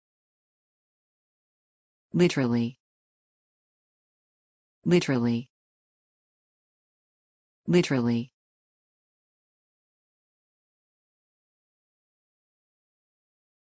How to say literally in English
A short video showing how to pronounce literally.